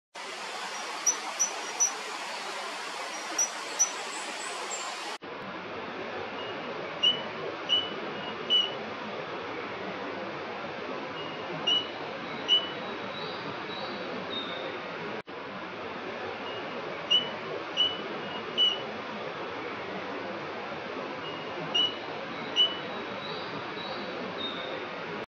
Tiny cricket, singing
Tiny (approx. 1 cm height) was singing and moving through vegetation by a river, possibly to attract a mate.

At The Lost Tinamou nature reserve @ Finca La Gracia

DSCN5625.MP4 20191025